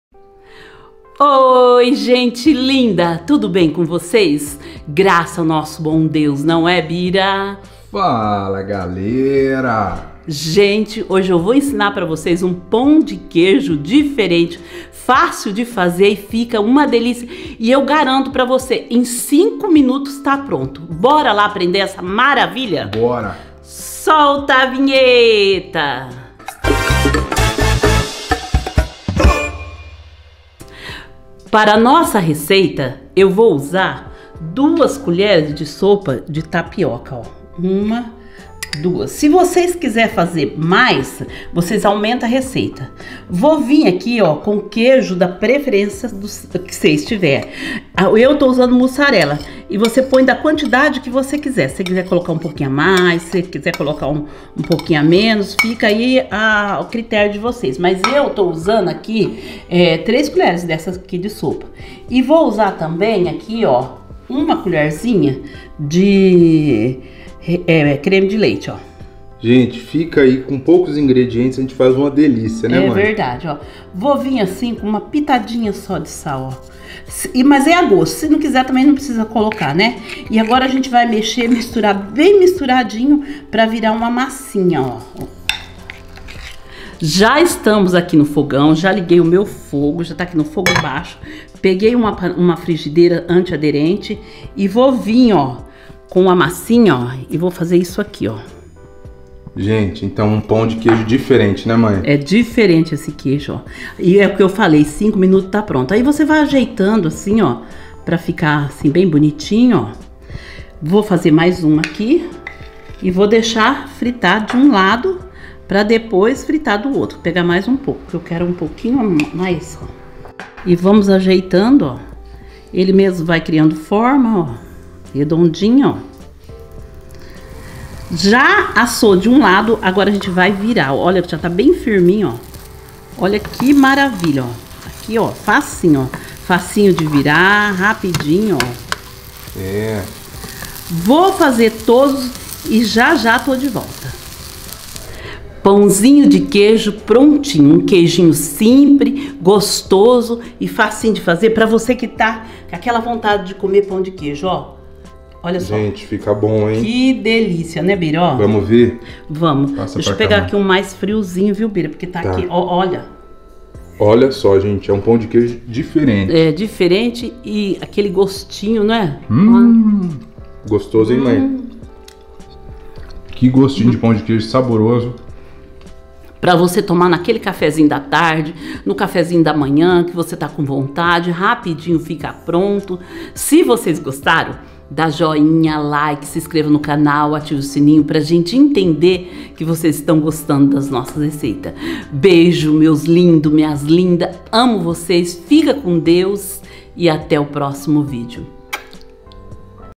PÃO DE QUEIJO DIFERENTE!! FEITO NO FOGÃO!! IGUAL ESSE VOCÊ NUNCA VIU!!
Oi gente linda!!

Canal do Bira e Bia (FALA GALERA)

Ingredientes

2 colheres (sopa) de tapioca
Queijo muçarela a gosto
1 colher (sopa) de creme de leite